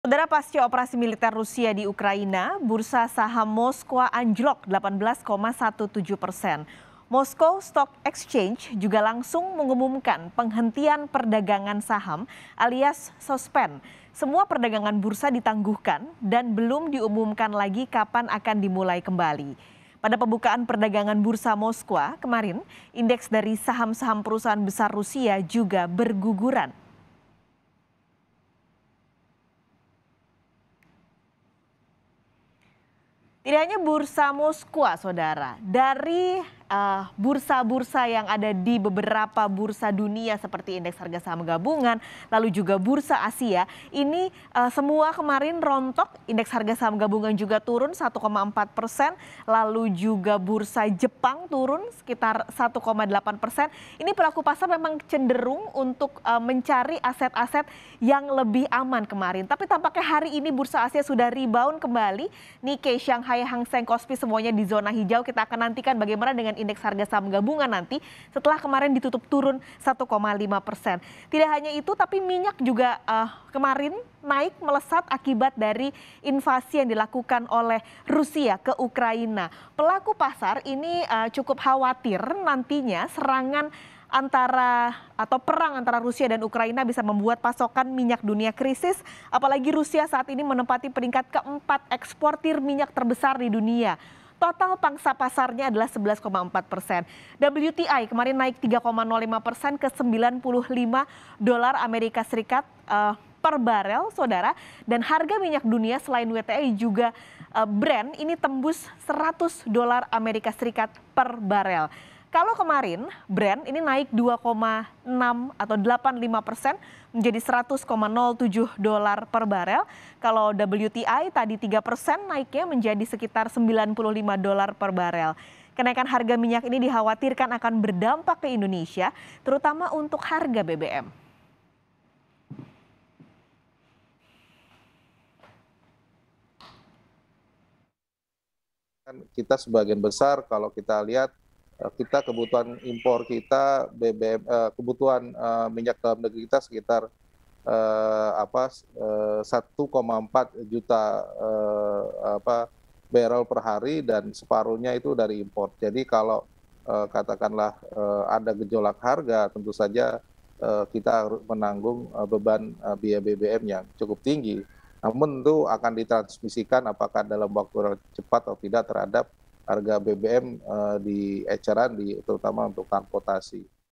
Dampak Invasi Rusia Ke Ukraina : Saham Moskow Disuspen, Hingga Melejitnya Harga Minyak Dunia!
JAKARTA, KOMPAS.TV - Pasca operasi militer Rusia di Ukraina, bursa saham Moskow anjlok 18,17 persen.

Moscow Stock Exchange juga langsung mengumumkan penghentian perdagangan saham, aliuas suspen.

Semua perdagangan bursa ditangguhkan dan belum diumumkan lagi kapan akan dimulai kembali.

Pada pembukaan perdagangan bursa Moskow, indeks dari saham-saham perusahaan besar Rusia juga berguguran.

Baca Juga Meski Harga Minyak Dunia Terus Melejit, Ekspansi Bisnis SPBU Akan Terus Berlanjut di

Harga minyak dunia juga akhirnya tembus angka USD 100 per barel.

Harga minyak jenis Brent, naik 2,85 persen menjadi 100,07 dollar per barel.

Sementara West Texas Intermediate melesat 3 persen menjadi 94,9 dollar per barel.

Kenaikan harga minyak ini dikhawatirkan akan berdampak ke Indonesia terutama untuk harga BBM.